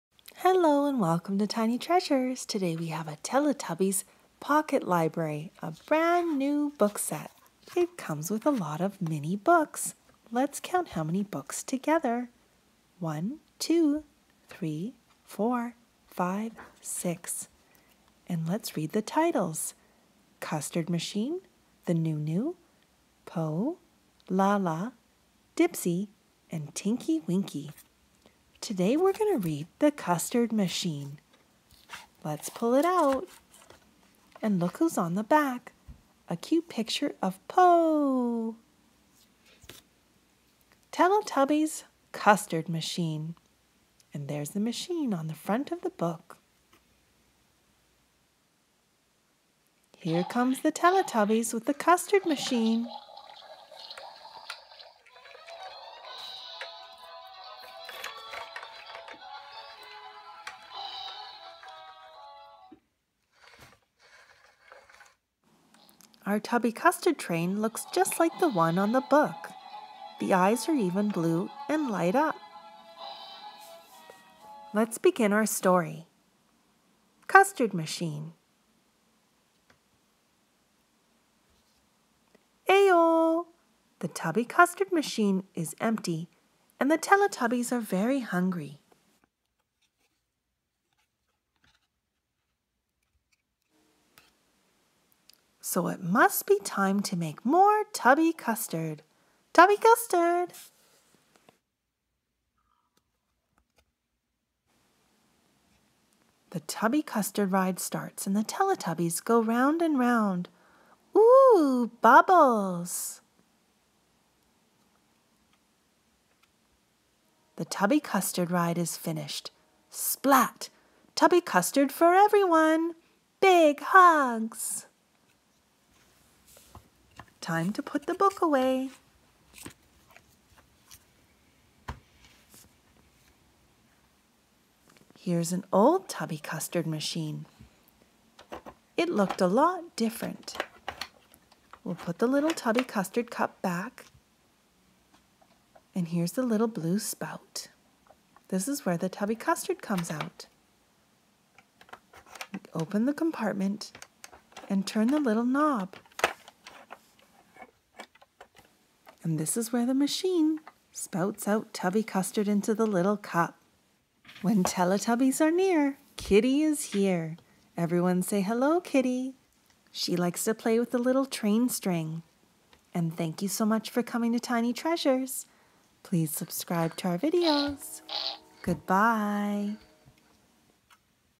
TUBBY CUSTARD Teletubbies Musical Toy and Read Along Mini Book!
Tiny Treasures reads a little book about Tubby Custard and the Teletubbies ride the Custard Train. Enjoy!